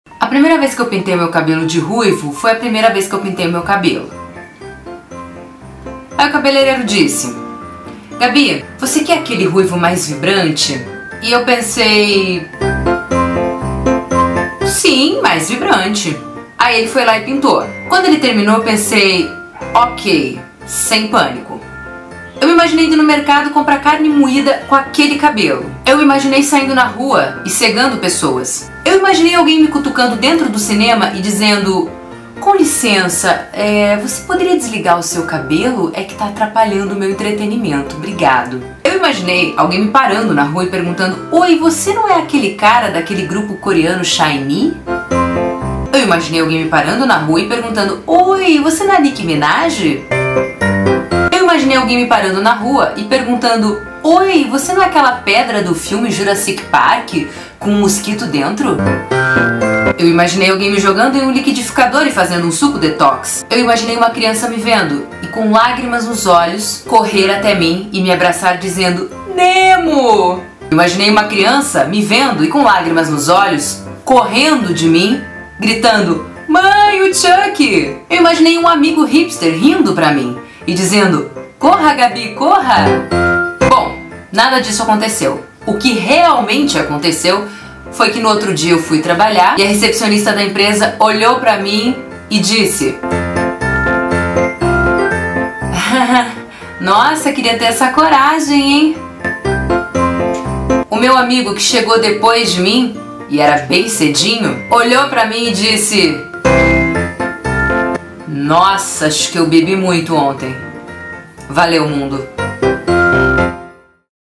Mudar gera crise
Quando você era morena e decide ser ruiva então, dá até crise de identidade.